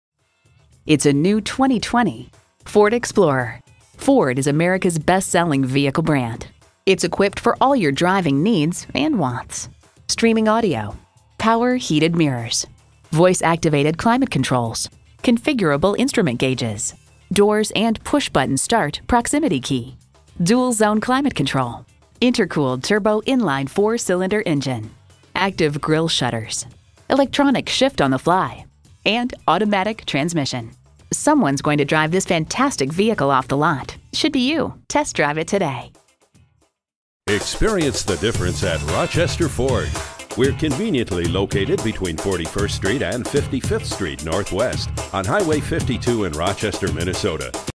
New 2020 Ford Explorer Rochester MN Winona, MN F205042 - SOLD
Year: 2020
Make: Ford
Model: Explorer
Trim: XLT
Engine: EcoBoost 2.3L I4 GTDi DOHC Turbocharged VCT
Transmission: 10-Speed Automatic
Color: Black
Interior: Ebony
Stock #: F205042
VIN: 1FMSK8DH2LGA62931

Address:
4900 Highway 52 North